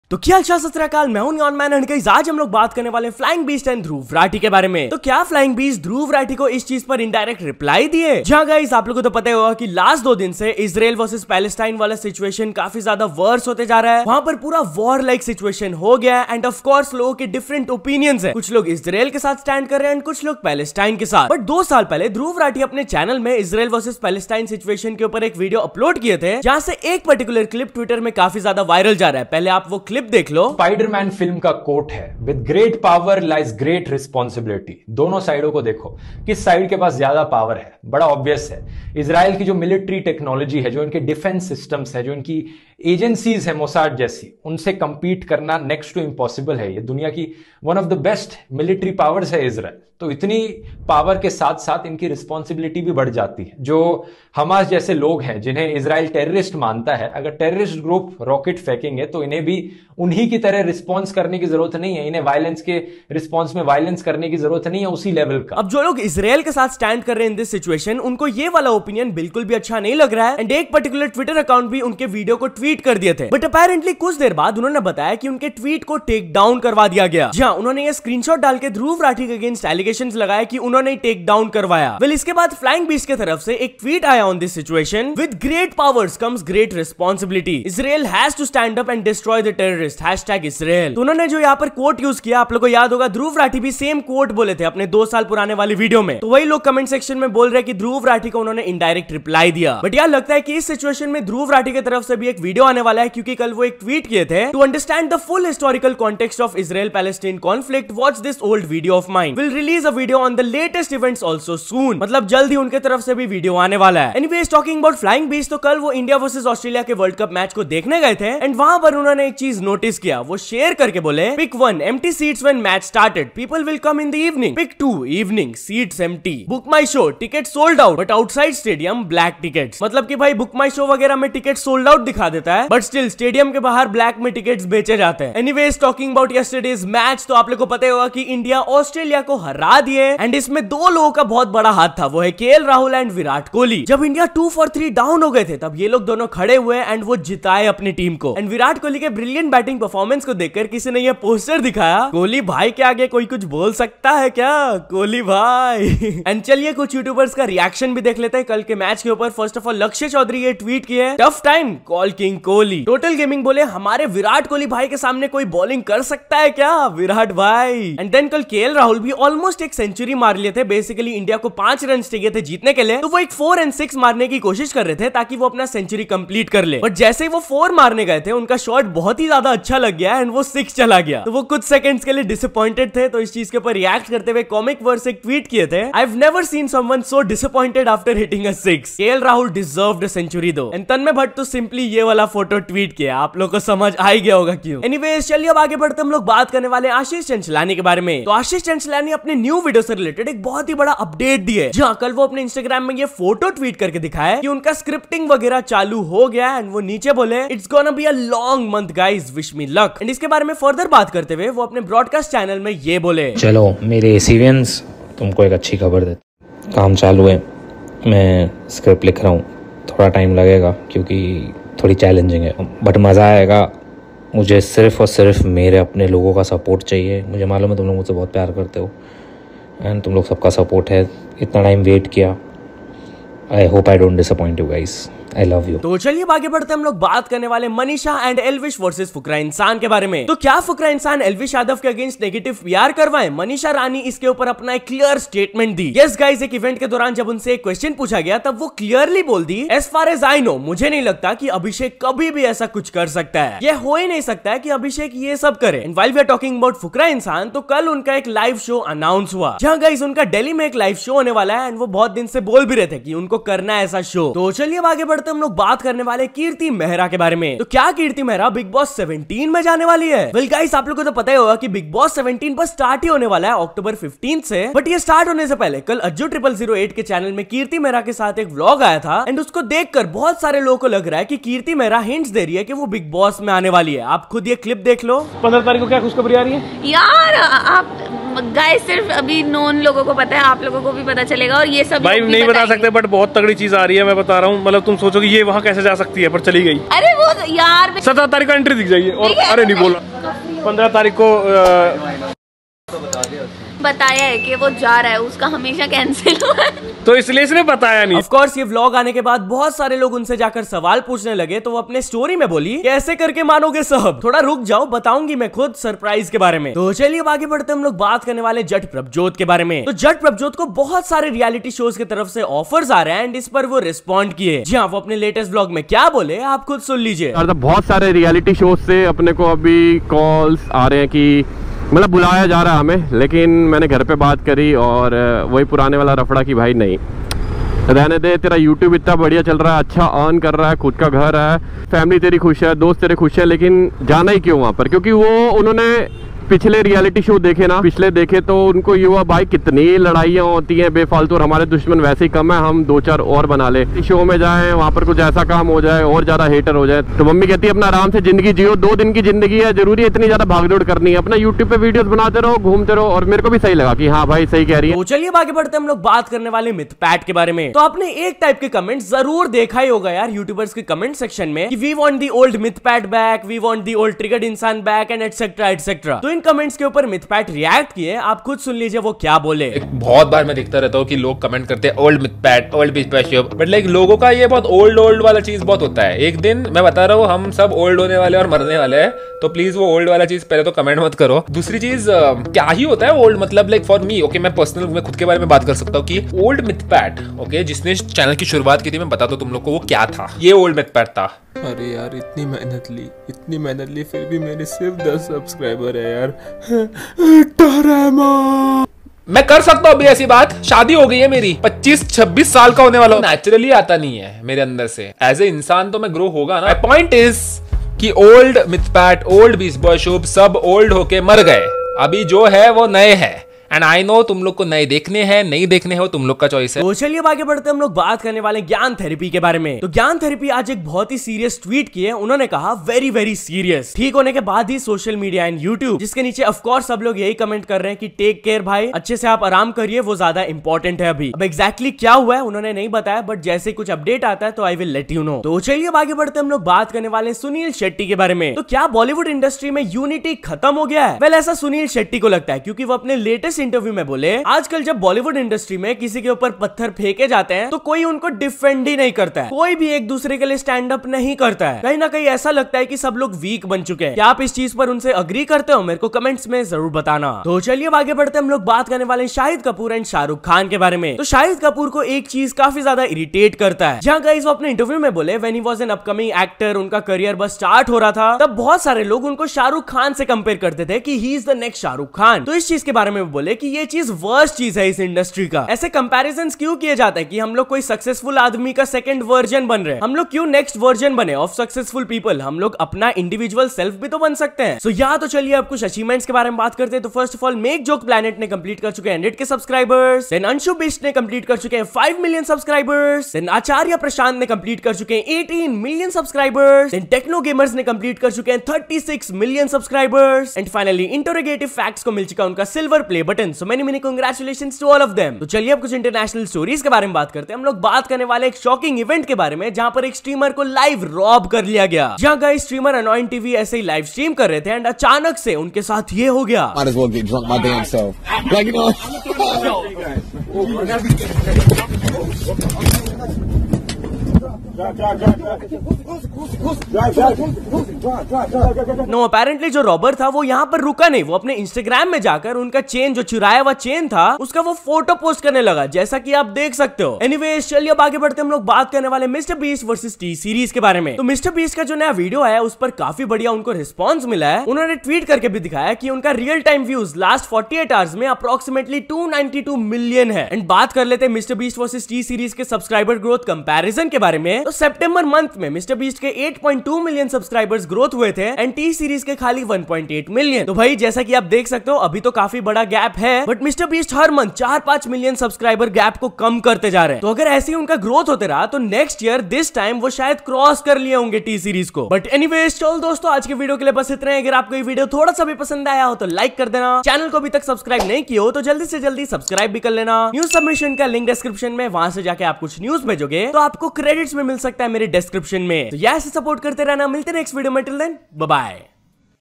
Flying Beast REPLY to Dhruv Rathee on this…?! | Virat Kohli & KL Rahul, Mythpat, MrBeast Vs T-Series
Today's topic includes Flying Beast & Dhruv Rathee. So, Flying Beast gave a reply to Dhruv Rathee?
Next,Flying Beast on World Cup Tickets being sold in Black.
Next,India Vs Australia Funny Virat Kohli poster.
Next,India Vs Australia reactions!
Next,Ashish Chanchlani new video.
Next,Manisha Rani says Fukra Insaan would never do Negative PR against Elvish Yadav!
Next,Fukra Insaan show.
Next,Kirti Mehra in Bigg Boss 17?
Next,Jatt Prabhjot gets Reality Show Offer!
Next,Mythpat on "We Miss Old Mythpat" comments.
Next,Gyan Therapy serious issue.
Next,No unity in Bollywood - says Suniel Shetty.
Next,Shahid Kapoor on comparisons with SRK.
Next,Make Joke Planet 100K, Anshu Bisht 5M, Acharya Prashant 18M, Techno Gamerz 36M, Interrogative Facts silver.
Next,Streamer robbed live!
Next,MrBeast huge growth Vs T-Series comparison!

0:00 Flying Beast Reply to Dhruv Rathee?!
1:54 Flying Beast Exposed World Cup Tickets
2:15 India Vs Australia Virat Kohli Funny Poster
2:36 India Vs Australia Reactions
3:16 Ashish Chanchlani New Video
3:57 Manisha Rani on Fukra Insaan Negative PR Controversy
4:15 Fukra Insaan Show
4:25 Kirti Mehra in Bigg Boss 17?
5:18 Jatt Prabhjot in Reality Shows?
6:06 Mythpat Unhappy with These Comments!
7:08 Gyan Therapy Serious Issue
7:26 No Unity in Bollywood?!
7:48 Shahid Kapoor Unhappy with this!
8:16 Achievements
8:37 Streamer Robbed Live
9:08 MrBeast Vs T-Series